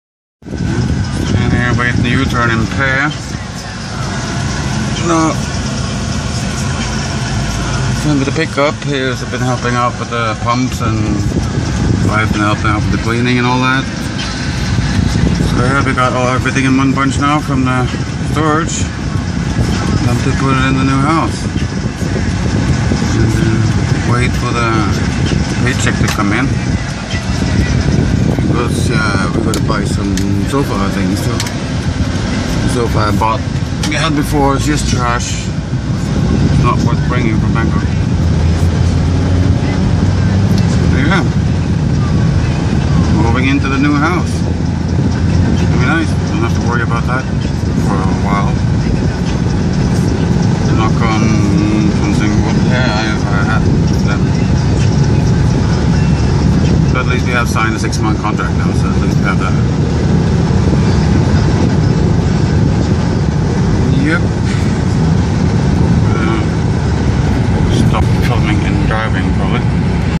Got all the stuff from storage. Moving in.
It's all getting sorted. Ready to move inn, just got to sort out some beds and sofa/chairs.